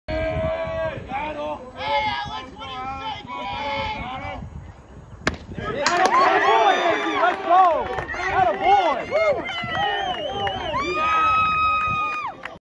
Andrew Shosho Works Out of a Bases Loaded Jam
After East Lyme loads the bases with no outs in the top of the 1st inning, Andrew Shosho bears down and holds the Vikings scoreless, including this strikeout to end the inning.